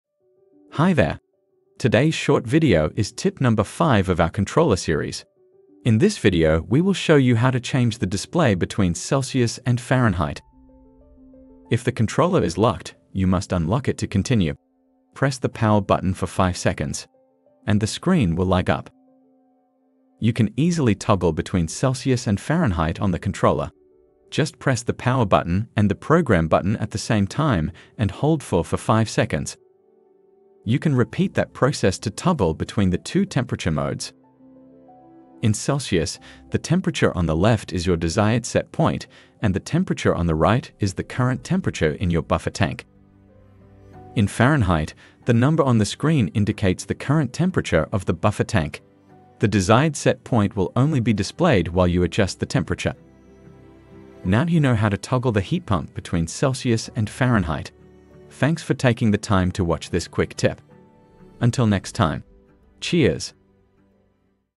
Arctic Heat Pumps - EVI Controller Part 5
Part 5 Instructional video that details the setup of an Arctic EVI heat pump controller.